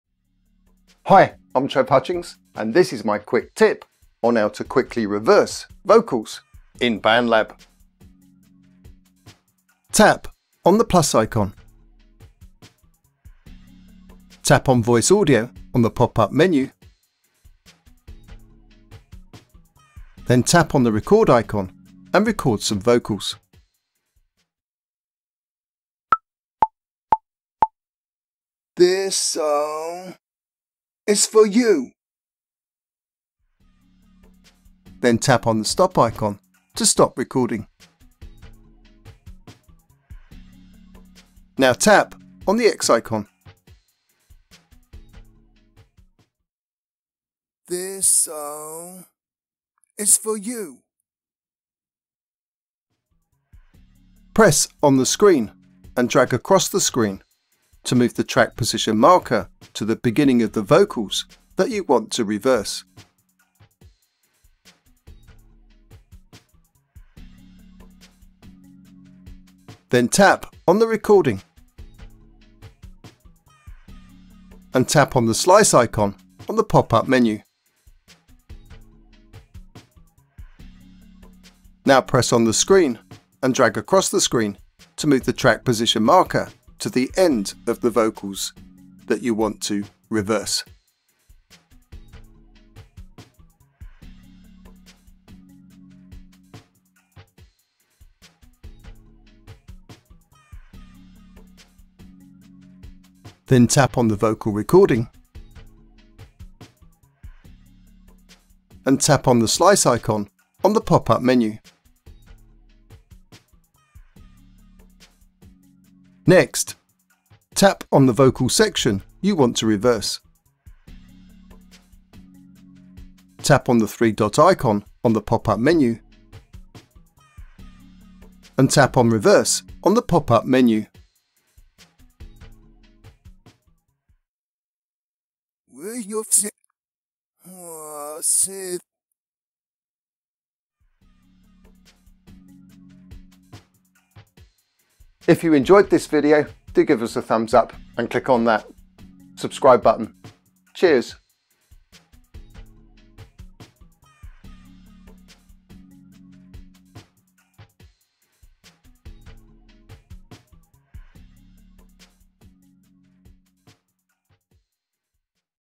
Bandlab Quick Tip... How to quickly reverse vocals
How to quickly reverse a section of recorded vocals on a vocal track in Bandlab.